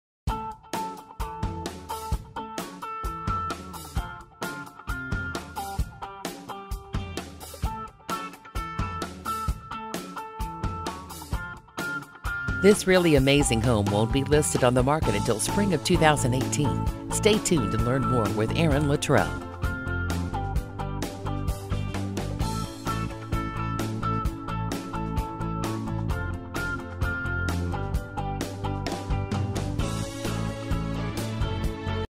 Aaron Luttrell presents 5370 S Washington St Littleton, CO | $0 | coloradohomes.com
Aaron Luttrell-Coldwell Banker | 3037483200

Really amazing home. Won't be listed and on the market until Spring of 2018! Ordering photos now bc it has a lovely yard in the summer!